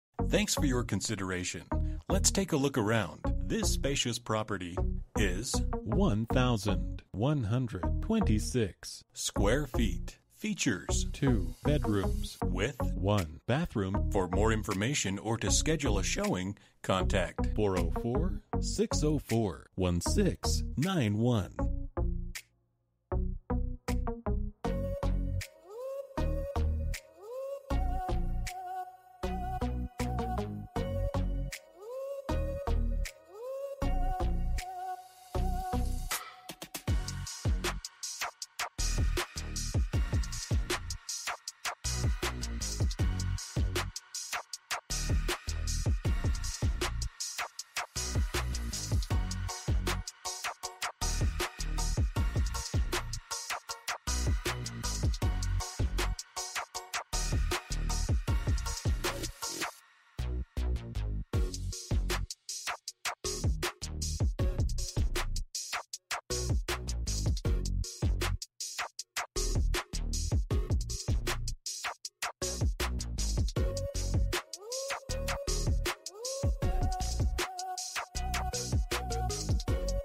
Residential for sale in Sandy Springs, GA - 304 Natchez Trace
Beautiful 2-bedroom Condo in such a convenient location! Quiet wooded community but minutes from everything, this 2nd floor unit has an Open Floorplan, Wood Floors, Granite Counters, Stainless Gas Range, Electric Fireplace with Remote Control, a refreshing Sun Room and your own private Rear Balcony. Carpeted Spacious Bedrooms with large closets (Walk-in Closet in the Main Bedroom), Bathroom with gorgeous Quartz Countertop & new Faucet. Large Laundry Room with Washer & Dryer included. Quiet well-run community. The HOA fees cover water, sewer, internet & basic cable. Takes 2 minutes to get on GA-400 and a couple minutes more to I-285. You can literally walk to Island Ford Park & the Chattahoochee River National Recreation Area, or Roswell Rd & Perimeter Mall area for some great shopping & dining just minutes away. This community also has ample parking, and there are no rental restrictions. This condo will move fast, hurry to see it.

Bedrooms: 2
Bathrooms: 1
Square Feet: 1126
Price: $225,000